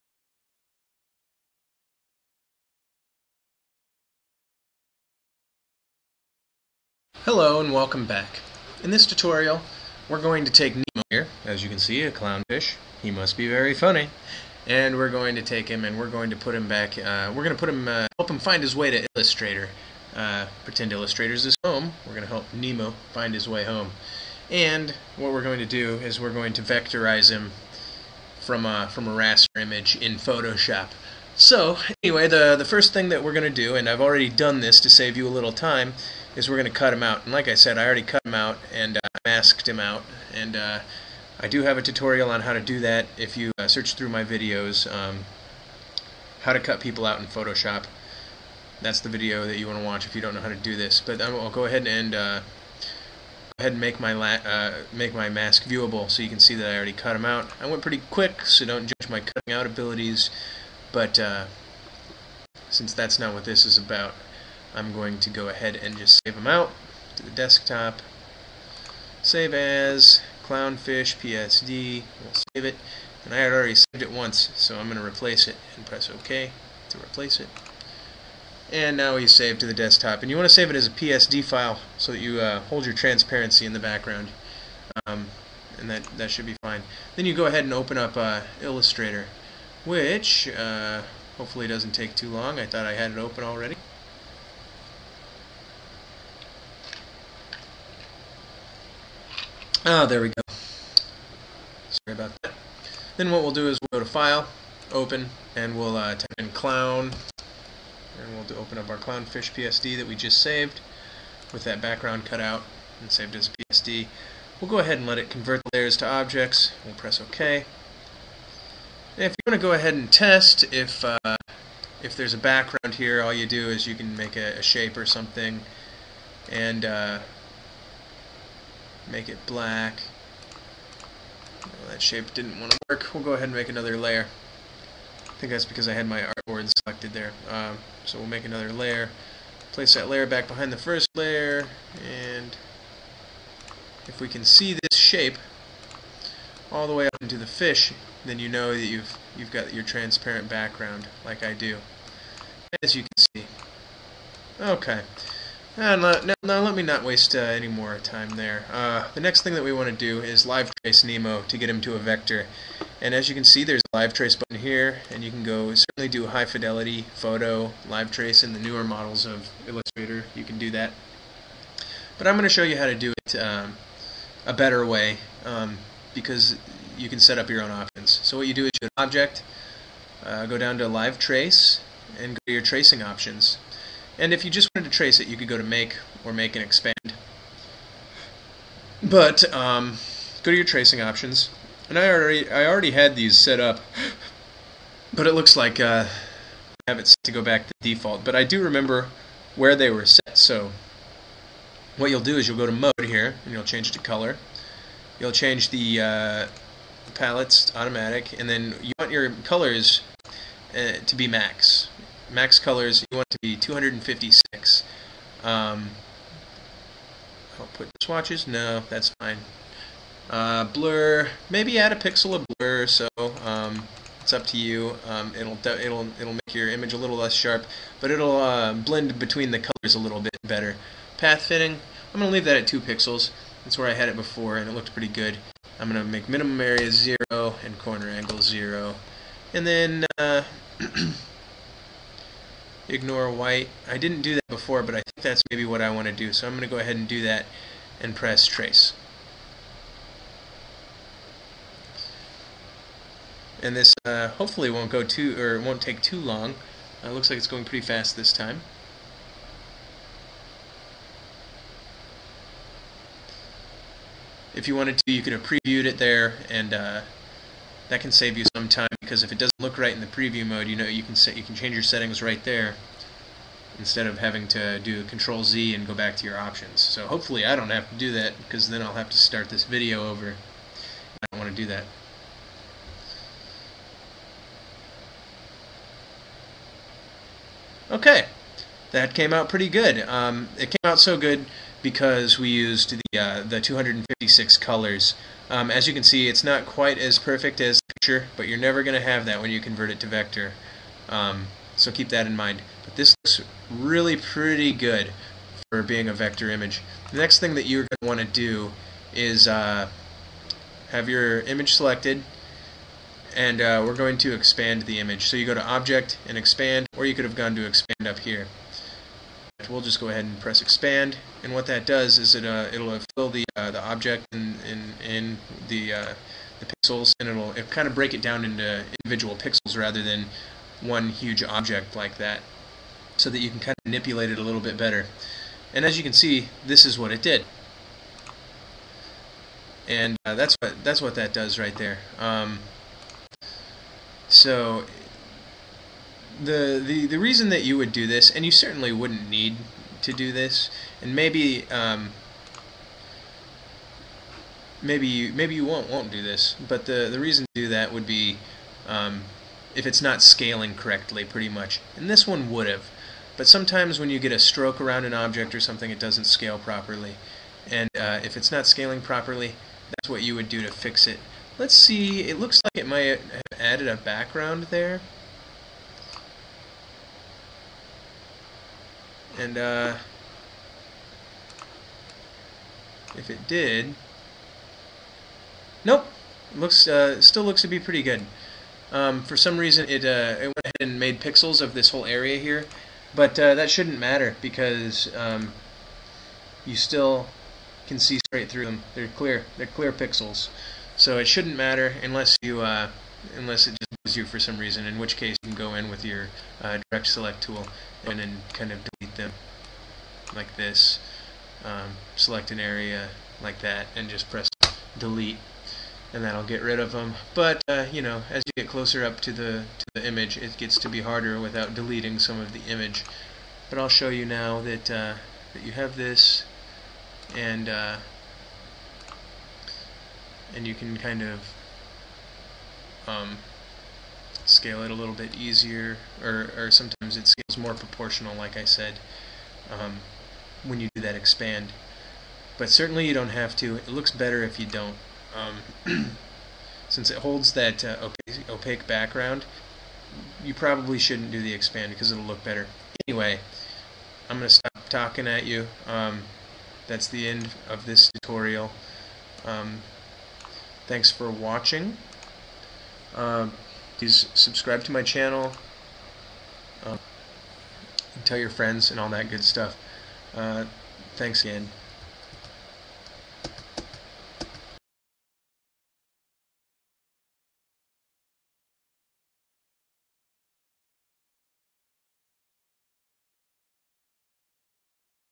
FIND NEMO Adobe Illustrator - convert raster to vector | Glazefolio Design Blog
Get Adobe Creative Cloud Photography plan with 20GB storage | 1 Year Subscription (Download)

Glazefolio Design Blog found Nemo in illustrator! Learn to convert images from rastor to vector in this tutorial video.